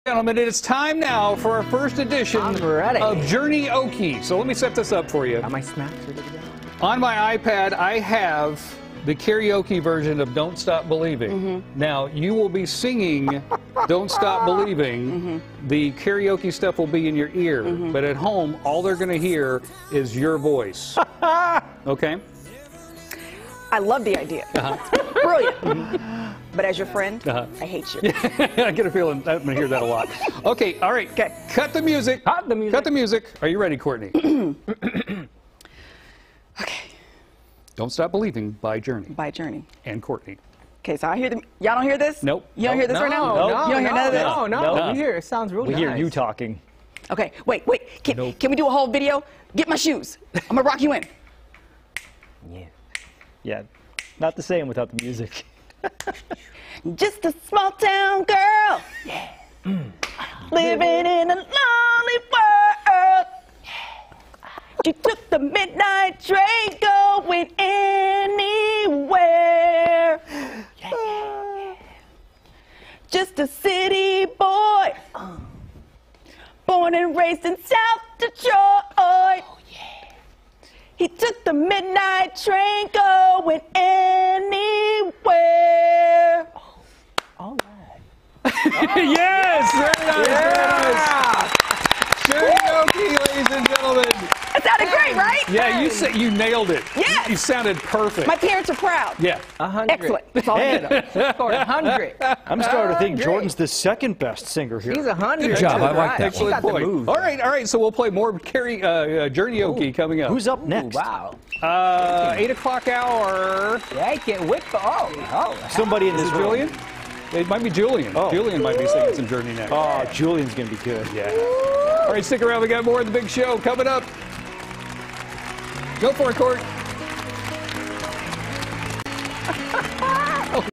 Journey-AOKE, 7am
Journey is playing at Golden 1 tonight, to celebrate we will be doing "Journey-aoke" - Courtney Dempsey is first!